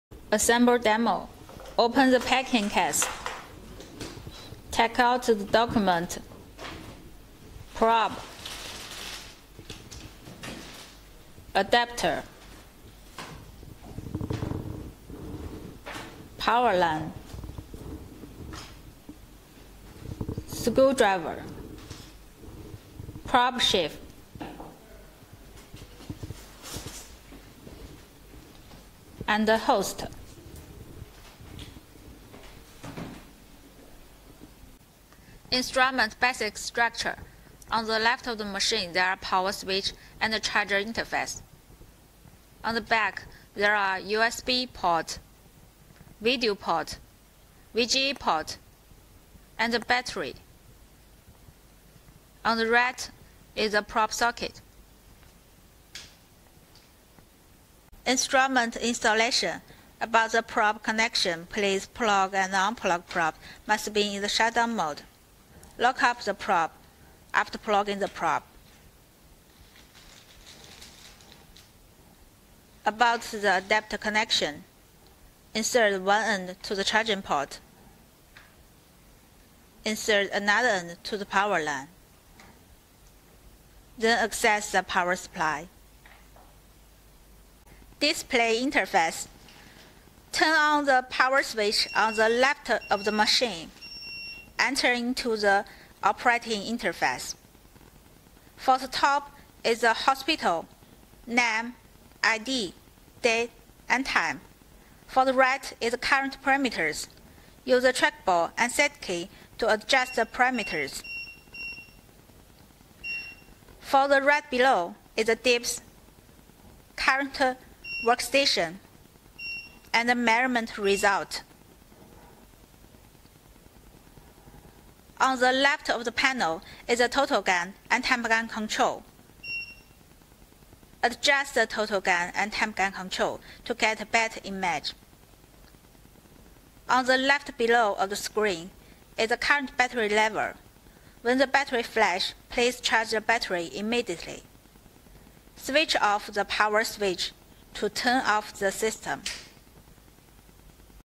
YJ-580 Version Full-Digital Laptop Ultrasound Scanner
Light and portable, much clearer image and convenient to operate. YJ-580 has an extra-long battery life with over 8 hours of standby time and 4 hours of working time - useful for consultation in various environment of cities, towns, outdoor. A variety of charging to ensure the examination consultation under different environments.
* 12.1 inch HD LCD display
* Rechargeable battery
* THI and Histogram function
* Support OB Report
*Support: Chinese/English/Spanish/French/Portuguese/Russian